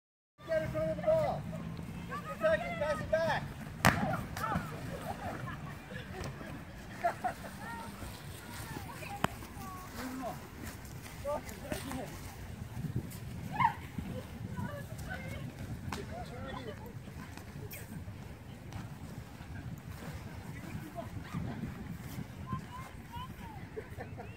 Twin Cities graduation party idea
Bubble Soccer, the best Minnesota graduation party idea. Bubble Soccer Minnesota providing event entertainment in the twin cities for yet another type of function.